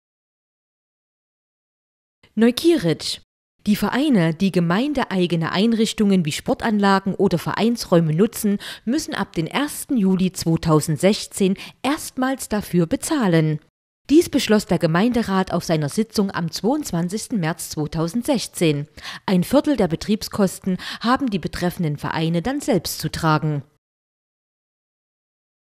Kurz Nachricht: Neukieritzscher Vereine müssen erstmals für Einrichtungen bezahlen
Neukieritzsch.

Neukieritzscher Vereine, die gemeindeeigene Einrichtungen wie Sportanlagen oder Vereinsräume nutzen, müssen ab dem 1. Juli 2016 erstmals dafür bezahlen.

Dies beschloss der Gemeinderat hat auf seiner Sitzung am 22. März 2016.

Ein Viertel der Betriebskosten haben die betreffenden Vereine dann selbst zu tragen.